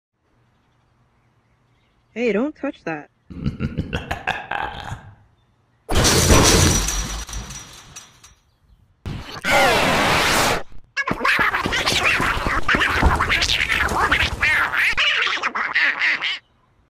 Natures Beauty Was Disrupted
Transfer student learns to not mess with goose.